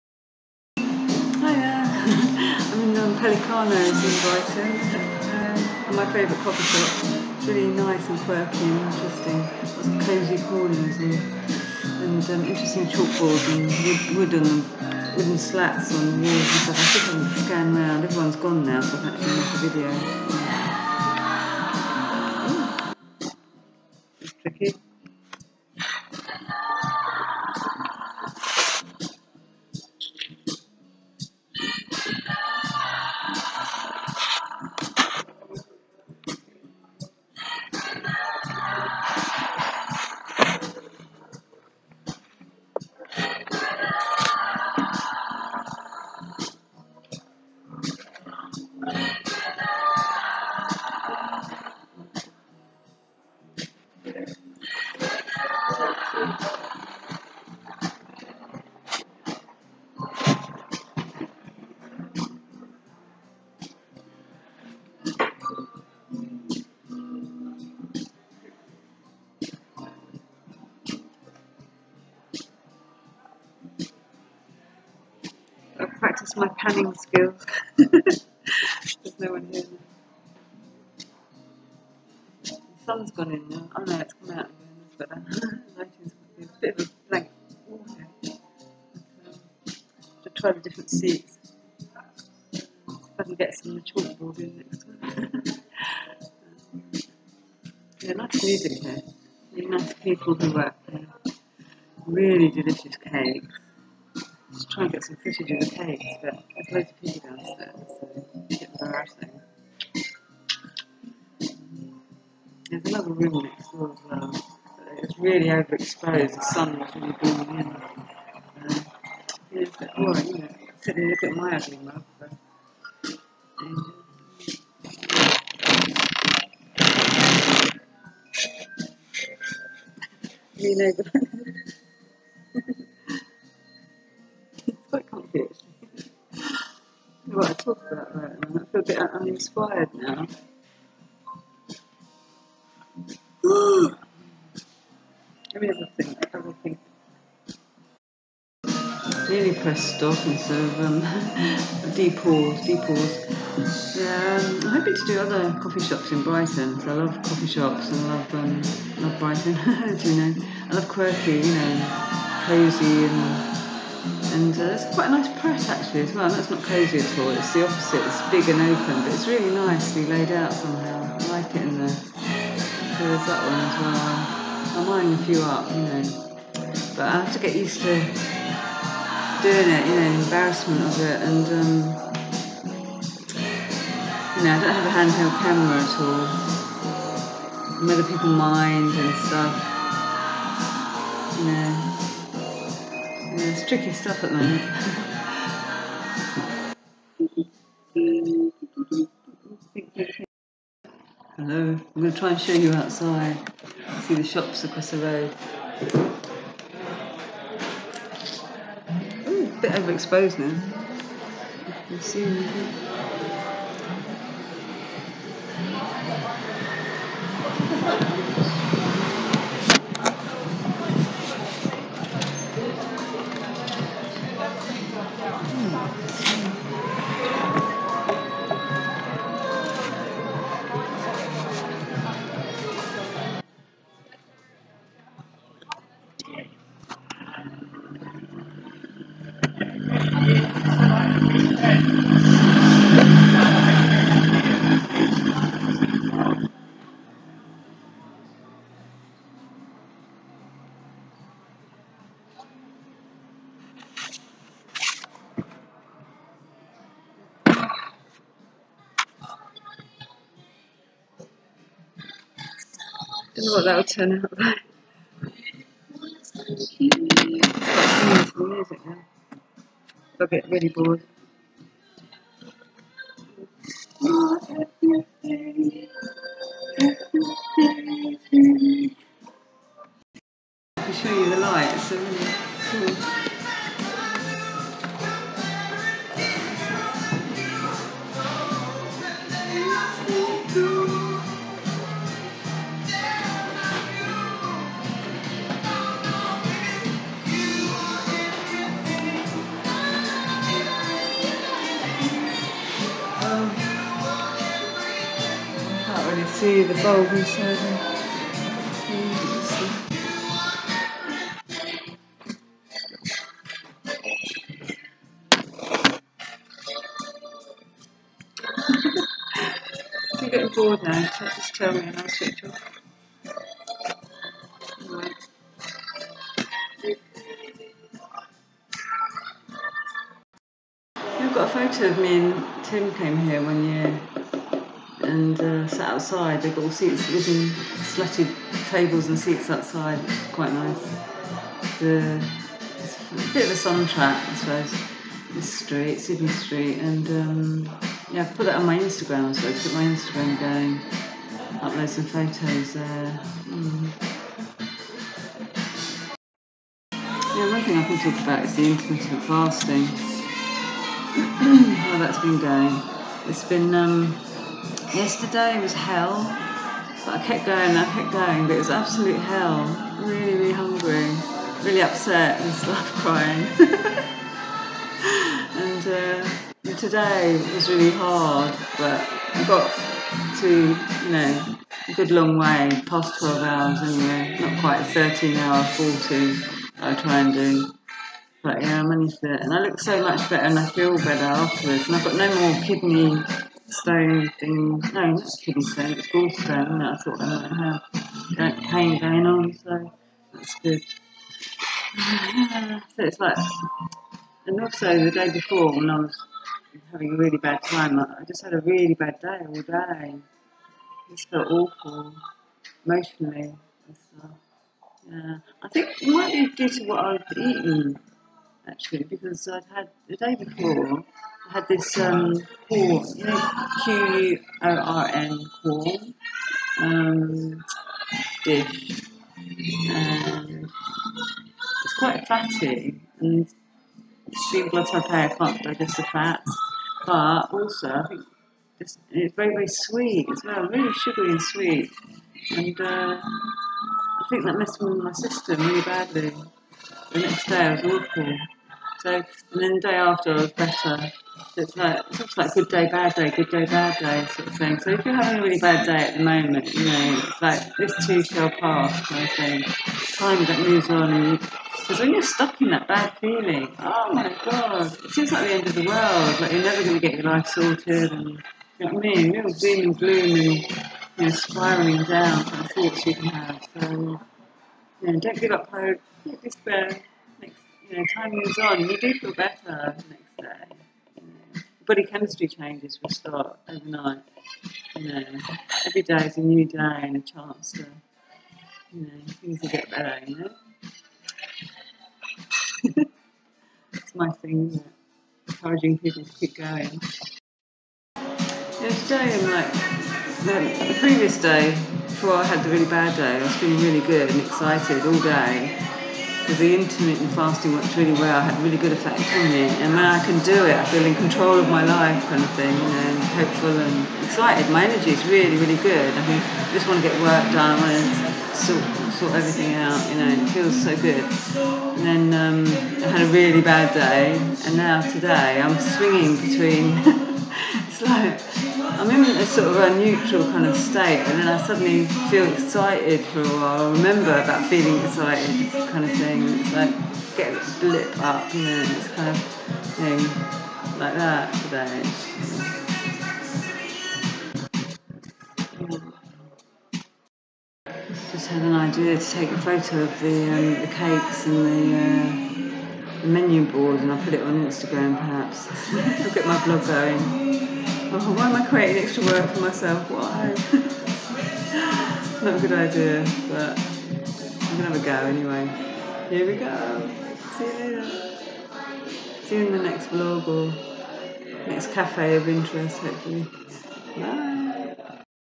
Pelicano cafe, Brighton - intermittent fasting talk
Upstairs at this unusual quirky cafe on Sydney Street, after 6pm.
The day I was there was the Thursday before Pride weekend so don’t know if that made it busier. I certainly saw a load of same sex couples in Waitrose the next day cosying up in the chiller cabinets excited for their weekend breaks or festitivies! Love dat. ❀
2:15: There were no desk seats in front of the more interesting chalk board, only the sofa which I tried but then I was too low, quite awkward.
2:20 Uninspired because I'd already spent about an hour in there before they all left! and it was after 6pm I think. Yeah, it stays open after 6pm, which I love! 7pm in fact - check that!!
2:45 In case you couldn’t hear, “Quite a nice Pret” (coffee/ sandwich shop: a large one in the centre of Brighton).
3:45 Bear with as I show you outside, I do get there in the end.
6:00 I finally think of something I want to talk about - intermittent fasting - how that's going. Don't give up if you feel bad. Time moves on and you do feel better. Body chemistry changes.
I downloaded Instagram for desktop Windows 10 and everything, but still doesn’t give me uploading rights ! I can’t load anything onto it at the mo, and my phone is already full up. It won't handle another App.